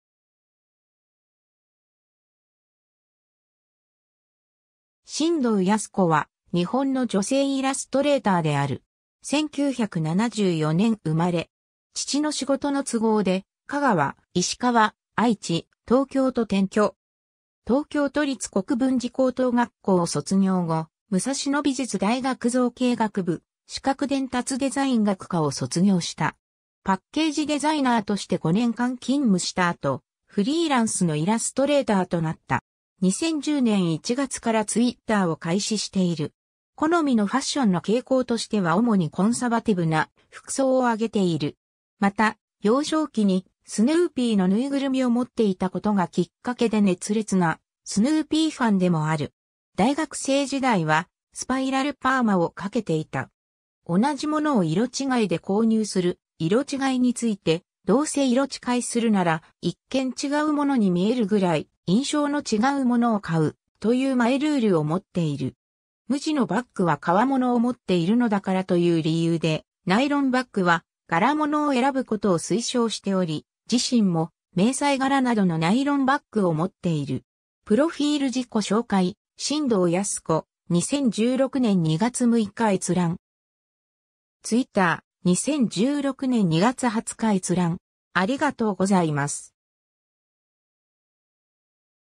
進藤やす子
進藤やす子（しんどう やすこ、1974年 - ）は日本の女性イラストレーターである。
 1974年生まれ。
父の仕事の都合で香川、石川、愛知、東京と転居。
東京都立国分寺高等学校を卒業後、武蔵野美術大学造形学部視覚伝達デザイン学科を卒業した。
パッケージデザイナーとして5年間勤務した後フリーランスのイラストレーターとなった。
2010年1月からTwitterを開始している。
 好みのファッションの傾向としては主にコンサバティブな服装を挙げている。
また、幼少期にスヌーピーのぬいぐるみを持っていたことがきっかけで熱烈なスヌーピーファンでもある。
大学生時代はスパイラルパーマをかけていた。
 同じものを色違いで購入する色チ買いについて、「どうせ色チ買いするなら一見違うものに見えるぐらい印象の違うものを買う」というマイルールを持っている。
無地のバッグは革モノを持っているのだからという理由でナイロンバッグは柄モノを選ぶことを推奨しており、自身も迷彩柄などのナイロンバッグを持っている。
 ^ profile 自己紹介進藤やす子、2016年2月6日閲覧。